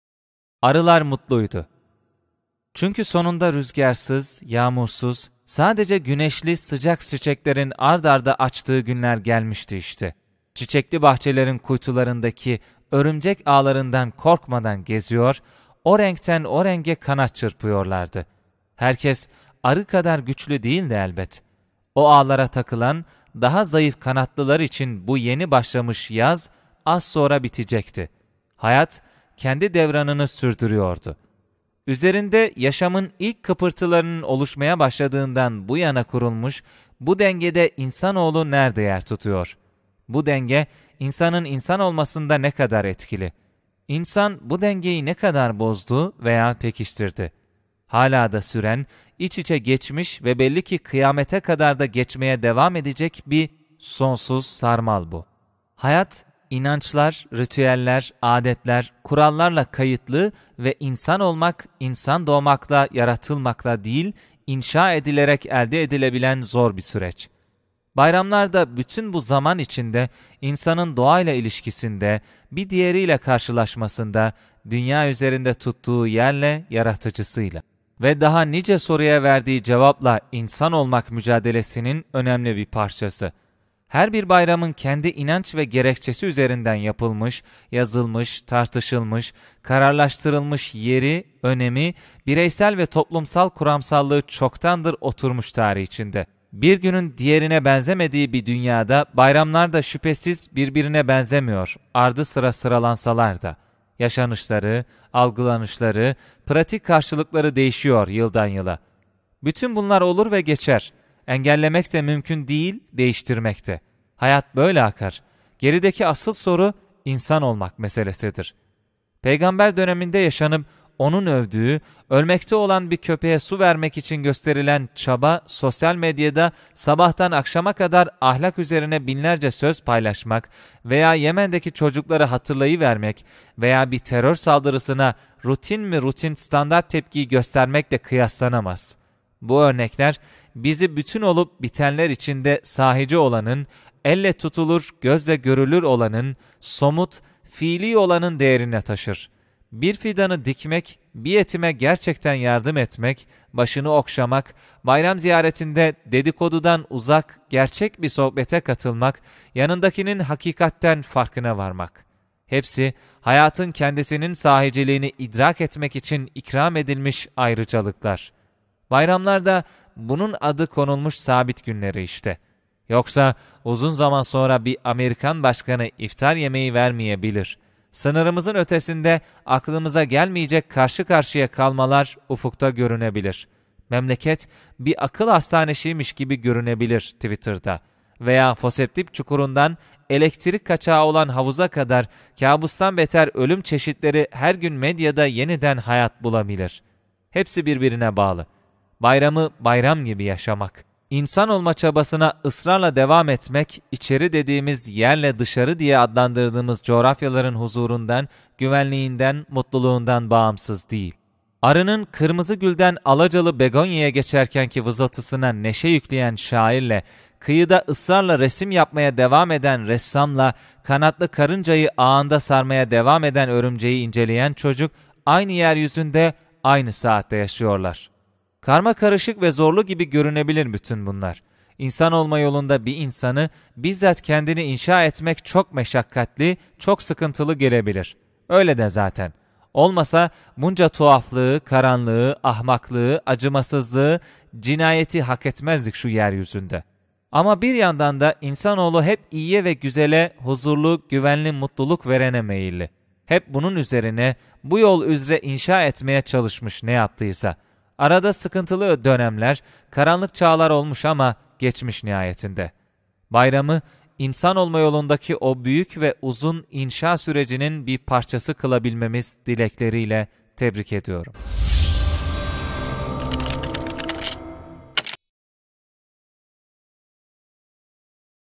Arılar mutluydu-Yaşar Taşkın Koç-27.06.2017
Çünkü sonunda rüzgârsız, yağmursuz sadece güneşli sıcak çiçeklerin art arda açtığı günler gelmişti işte. Çiçekli bahçelerin kuytularındaki örümcek ağlarından korkmadan geziyor, o renkten o renge kanat çırpıyorlardı. Herkes arı kadar güçlü değildi elbet; o ağlara takılan daha zayıf kanatlılar için bu yeni başlamış yaz az sonra bitecekti… hayat kendi devranını sürdürüyordu.